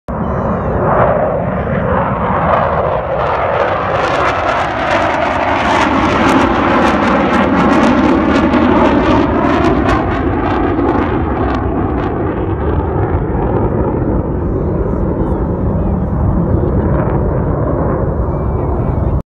VFA-86 Sidewinders
A pair of Super Hornets with VFA-86 Sidewinders out of NAS Lemoore at Mesa Gateway (formerly Williams AFB)